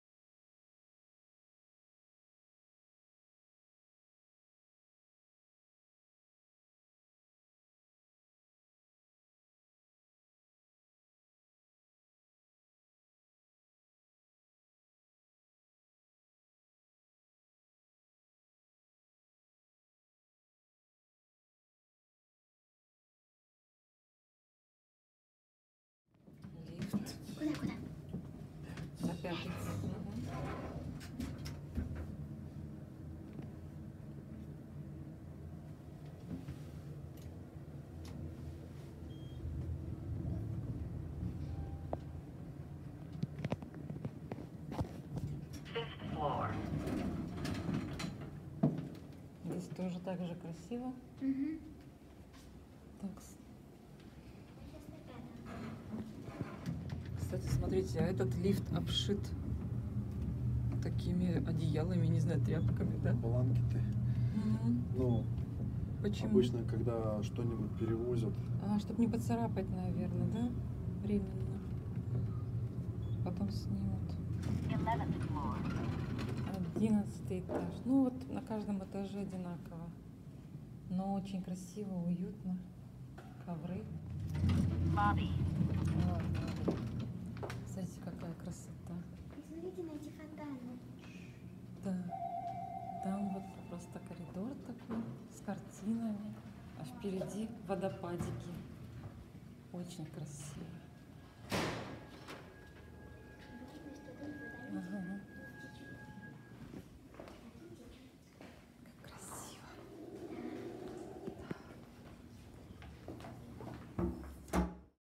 США | ПРОСТО ДОМ ДЛЯ ПОЖИЛЫХ | КАК ЖИВУТ ПЕНСИОНЕРЫ в Америке
❤️ Поделись, пожалуйста, ссылкой с тем, кто ищет работу.

✔ ДЕТСКИЙ КАНАЛ 🎈Eva from America 🎈